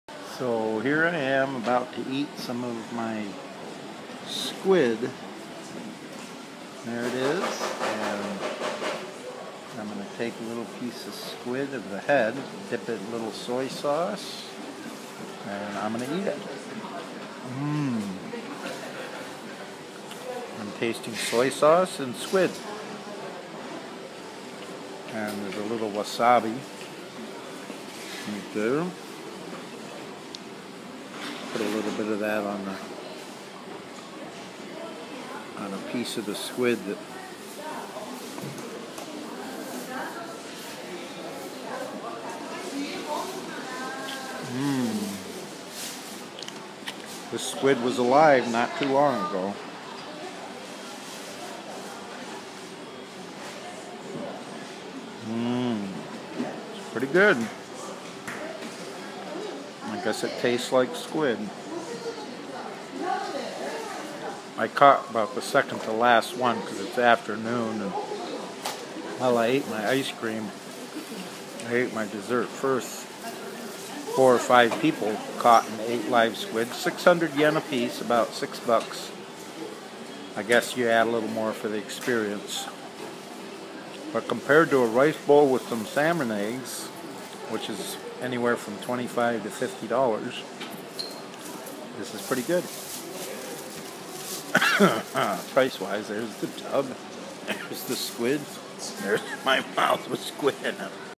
Eating squid
Me eating my live caught squid in Hakodate , Hokkaido, Japan April, 2014